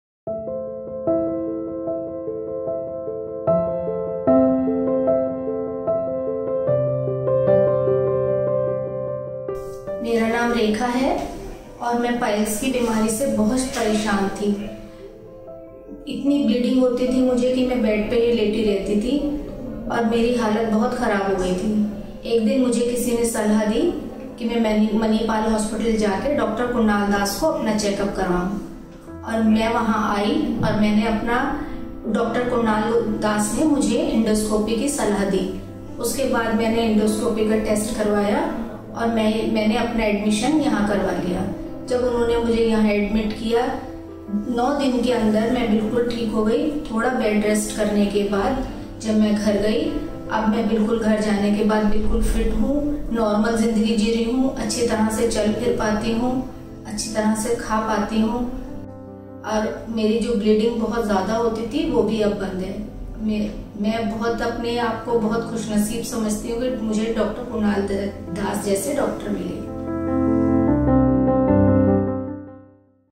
Hemorrhoids Treatment | Piles Disease Treatment | Dr. Kunal Das | Manipal Hospitals Delhi
Hear from Rekha, who underwent treatment for Hemorrhoids [Piles], at Manipal Hospitals Delhi. Facing intense discomfort, she chose to consult Dr. Kunal Das - HOD, Department of Gastrointestinal Sciences at Manipal Hospitals Delhi. After treatment, Rekha's condition has improved significantly and she has great relief. Watch the video to know about her experience undergoing treatment at Manipal Hospitals Delhi.